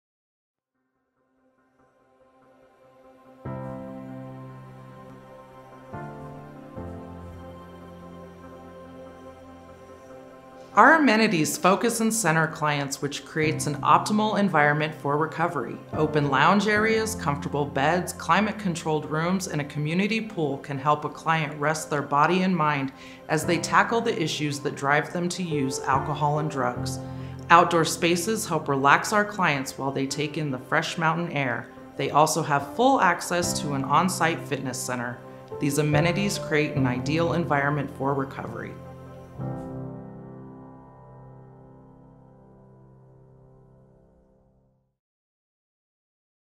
Our Amenities - Mountain Springs Recovery
Shannon, our Behavioral Health Technician, goes over the various amenities we have available at Mountain Springs, and how they can help to benefit your recovery.

To learn more about our facility and the amenities we have available, feel free to visit our